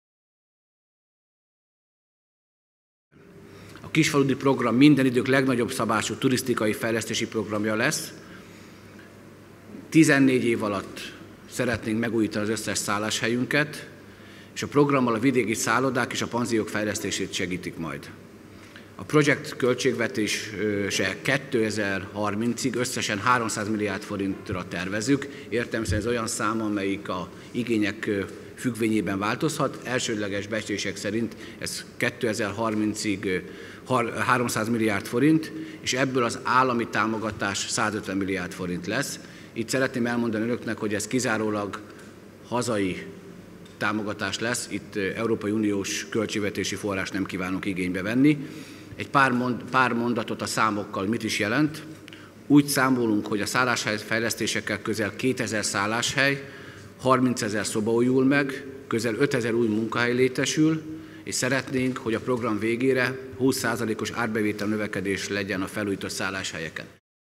Turisztikai fejlesztési program indul 300 milliárd forintos keretösszeggel
Mintegy 300 milliárd forintos keretösszeggel indul el a Kisfaludy turisztikai fejlesztési program, amelyben vidéki szálláshelyek fejlesztésére lehet pályázni 2030-ig - jelentette be Seszták Miklós nemzeti fejlesztési miniszter.